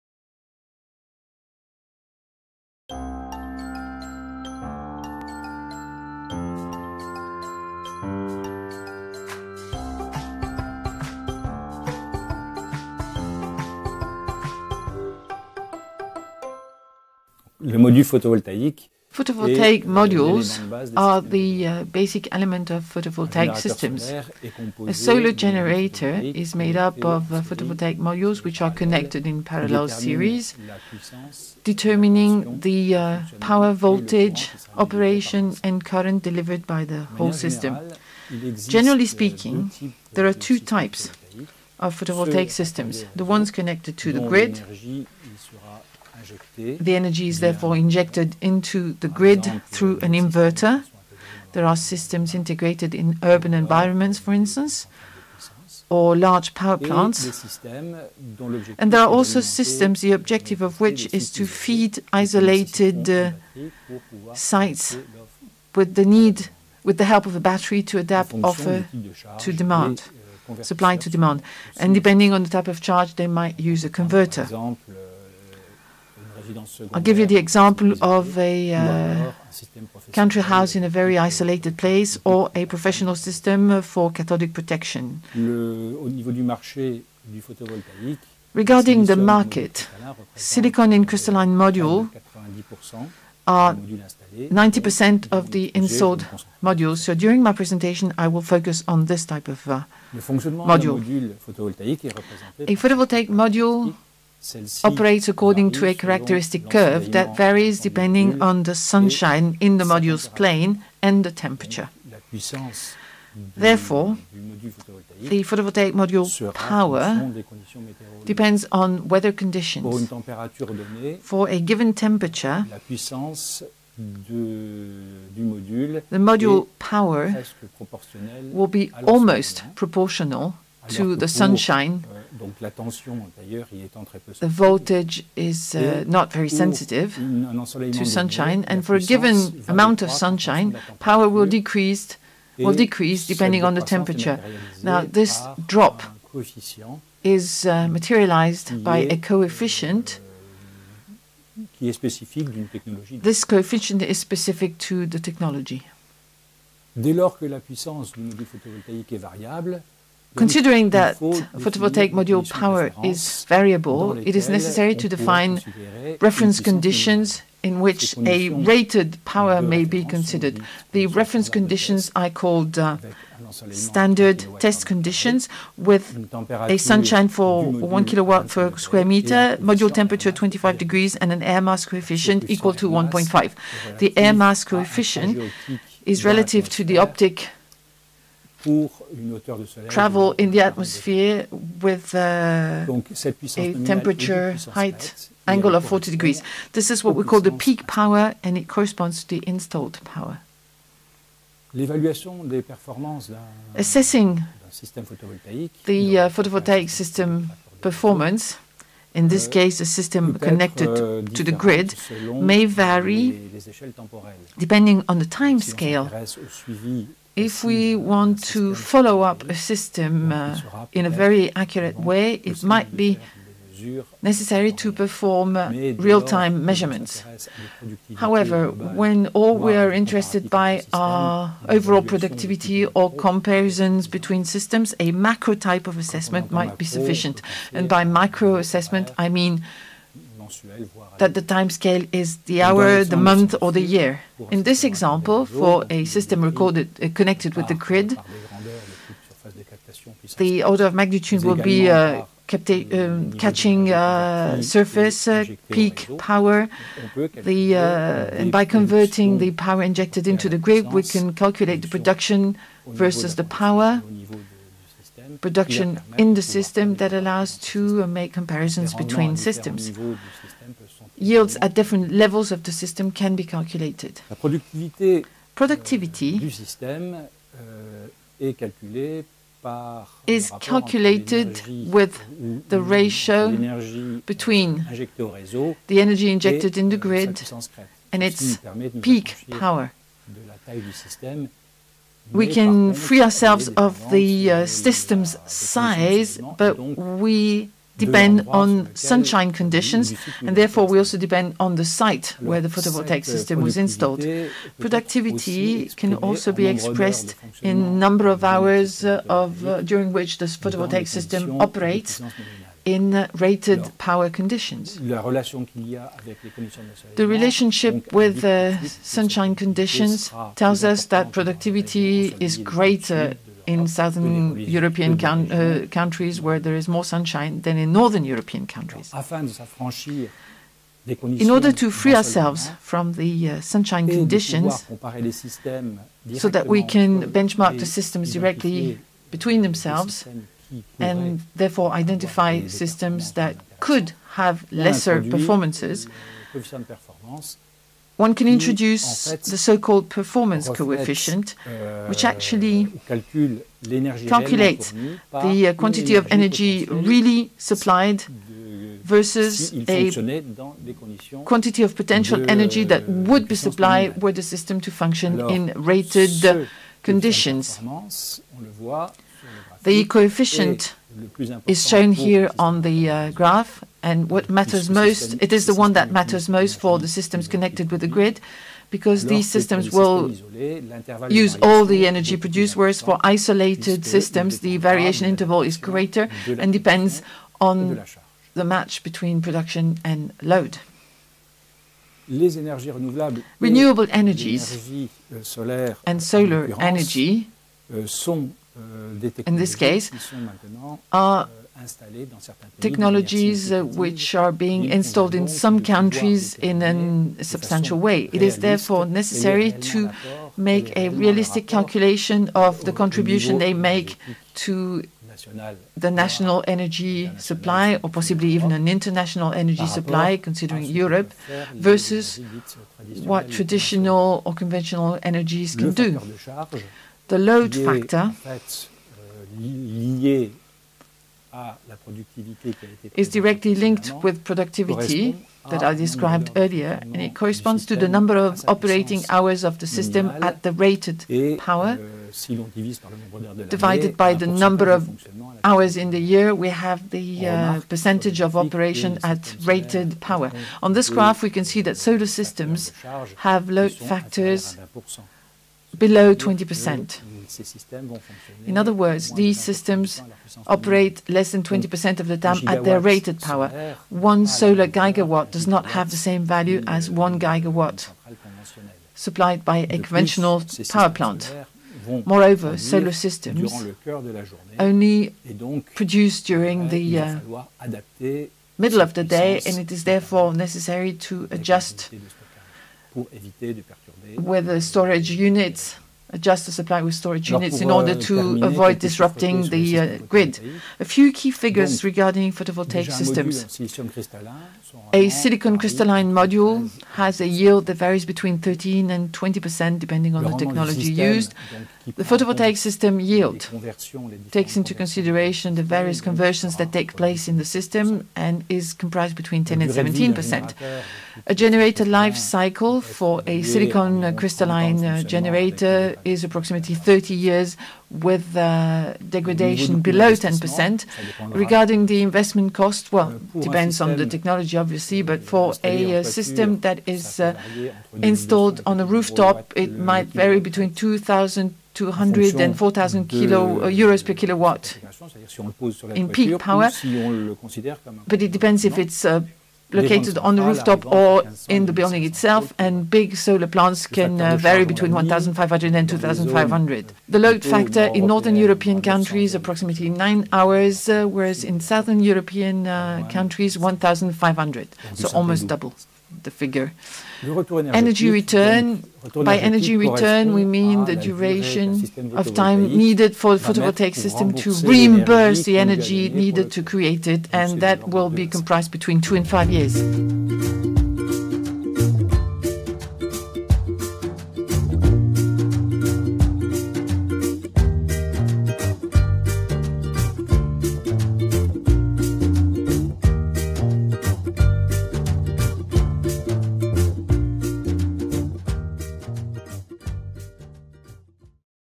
Solar energy - photovoltaic : the system's level of performance
In this video, Didier Mayer, Professor at MINES ParisTech, talks about the performance of photovoltaic systems. He relates it to factors such as sunshine and temperature, and introduces several essential characteristics such as load factor, coefficient of performance and system productivity.